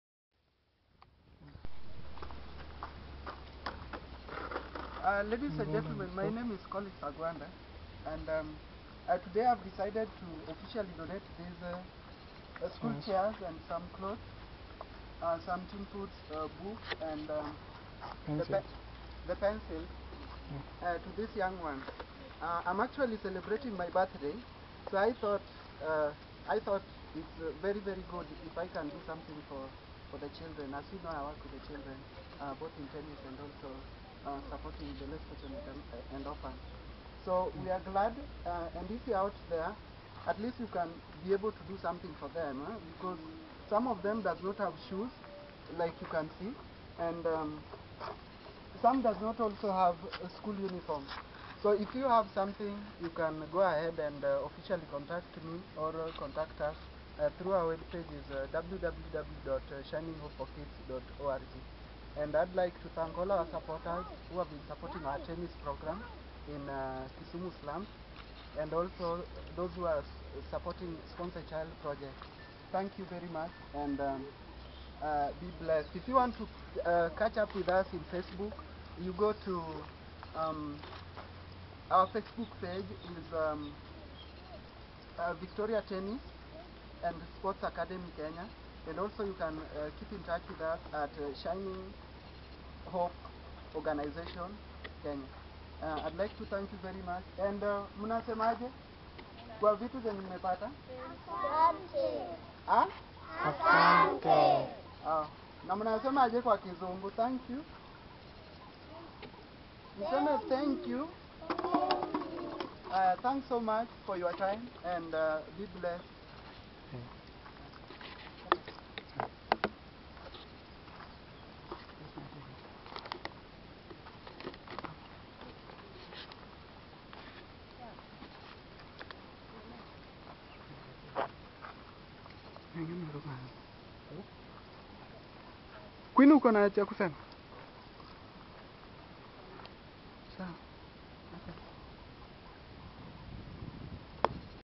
17th OCT 2013 DONATING SCHOOL MATERIALS TO ELORA SCHOOLS
On 17th October 2013, i, Collins Agwanda, Co-Founder of Elora Schools donated school materials to the orphaned and less fortunate kids we support at the school.